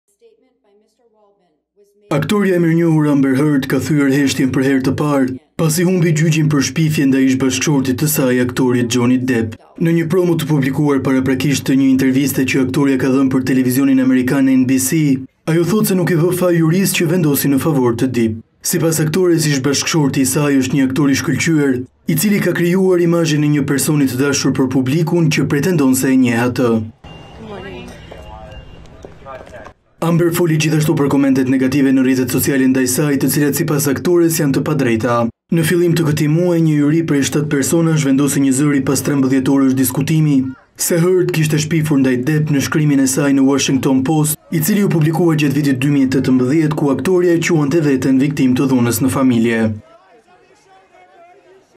Tv Klan - Amber Heard thyen heshtjen pasi humbi gjyqin | Lajme News
Aktorja e mirënjohur Amber Heard ka thyer heshtjen për herë të parë pasi humbi gjyqin për shpifje ndaj ish-bashkëshortit të saj, aktorit Johnny Depp.

Në një promo të publikuar paraprakisht të një interviste që aktorja ka dhënë për televizionin amerikan NBC, ajo thotë se nuk i vë faj jurisë që vendosi në favor të Dipp.

Sipas aktores, ish-bashkëshorti i saj është një aktor i shkëlqyer i cili ka krijuar imazhin e një personazhi të dashur për publikun, i cili pretendon se e njeh atë.

Amber foli gjithashtu për komentet negative në rrjetet sociale ndaj saj të cilat sipas aktores janë të padrejta. Në fillim të këtij muaji, një juri prej shtatë personash vendosi njëzëri pas 13 orësh diskutimi se Heard kishte shpifur ndaj Depp, në shkrimin e saj në Ëashington Post të vitit 2018 ku e quante veten viktimë të dhunës në familje.